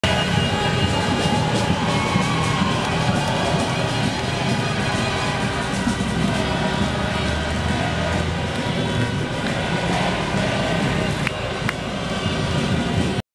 MVI_2415a.MOV
Festejo uruguayo por estar entre los 8 mejores del mundo en fútbol, 2010